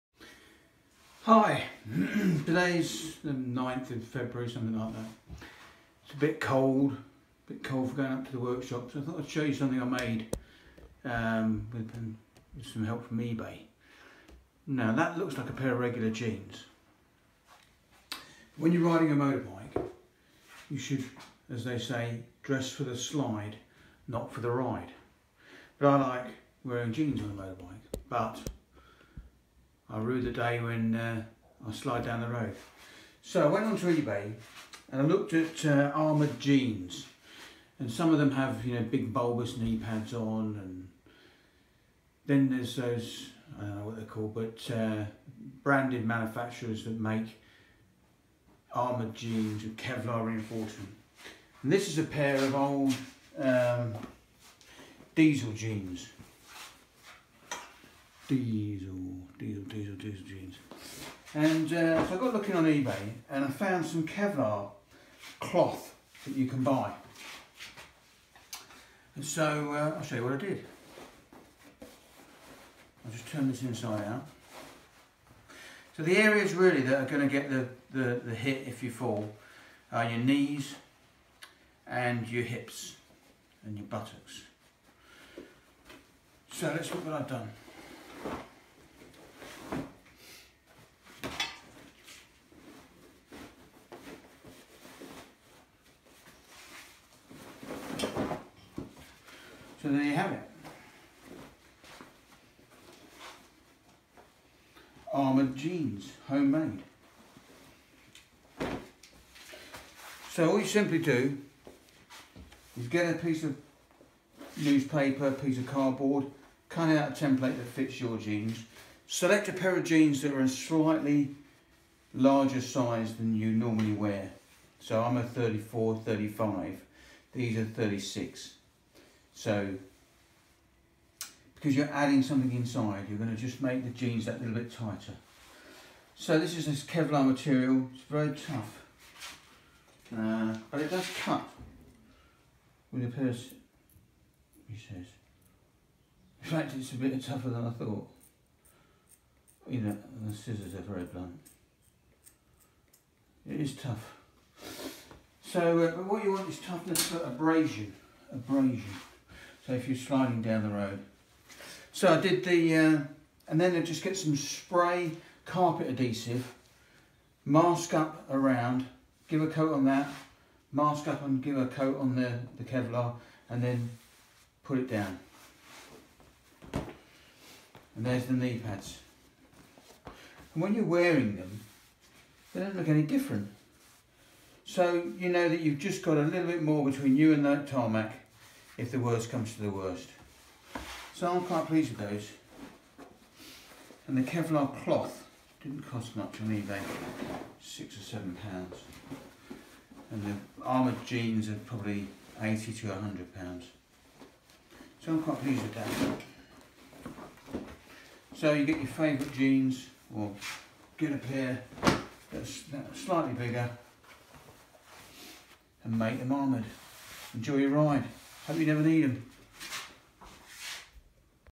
Motorcycle crash protection Armoured Jeans make your own
Make your own Armoured Jeans buy some Kevlar cloth from eBay and stick the panels inside your jeans with carpet adhesive